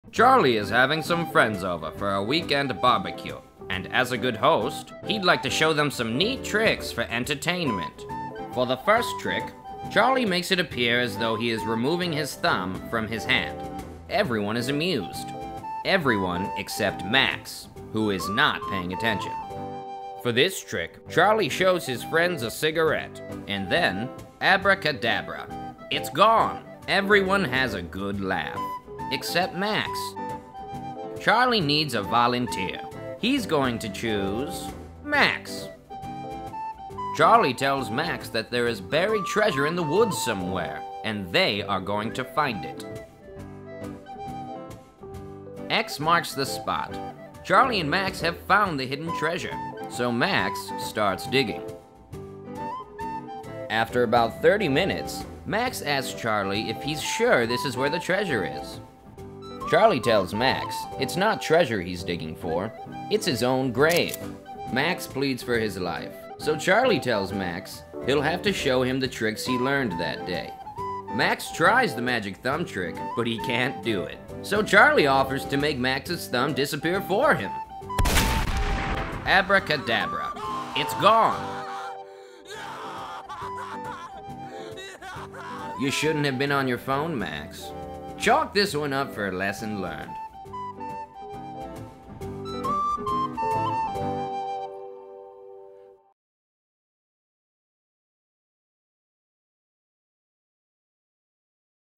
Fun Party Tricks
Charlie entertains his friends with some good ole' fashion trickery.

Special Thanks to Jesse Lesniak, Zach Maher, Matt Hawks, Jake Rohr, John Ricchiuto